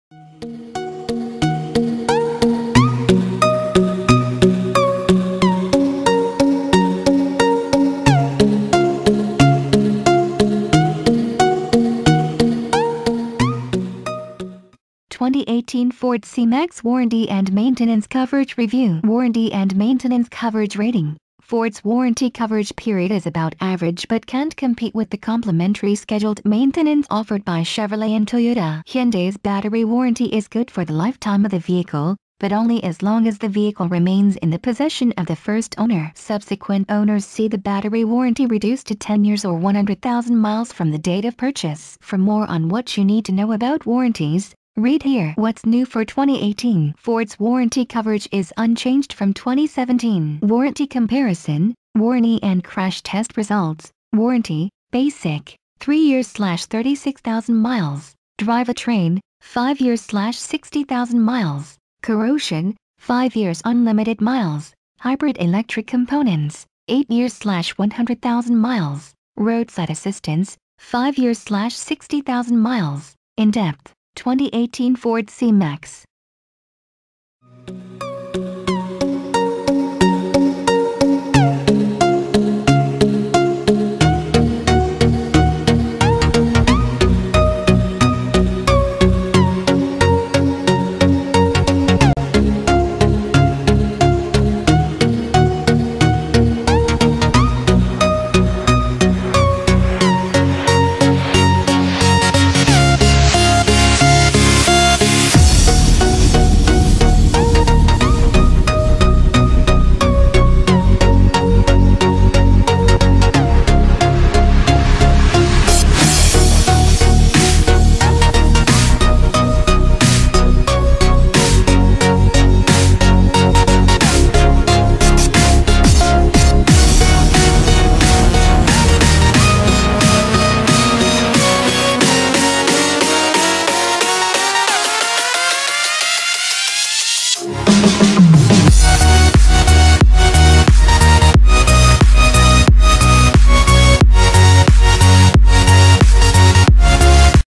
2018 Ford C Max Warranty and Maintenance Coverage Review
2018 Ford C-Max Warranty and Maintenance Coverage Review

Warranty and Maintenance Coverage Rating:
Ford’s warranty coverage period is about average but can’t compete with the complimentary scheduled maintenance offered by Chevrolet and Toyota. Hyundai’s battery warranty is good for the lifetime of the vehicle, but only as long as the vehicle remains in the possession of the first owner. Subsequent owners see the battery warranty reduced to 10 years or 100,000 miles from the date of purchase.

For more on what you need to know about warranties, read here.
Ford’s warranty coverage is unchanged from 2017.

Warranty Comparison
Warranty and Crash Test Results
Warranty
Basic:
3 Years/36,000 Miles
Drivetrain:
5 Years/60,000 Miles
Corrosion:
5 Years/Unlimited Miles
Hybrid/Electric Components:
8 Years/100,000 Miles
Roadside Assistance:
5 Years/60,000 Miles
In Depth: 2018 Ford C-Max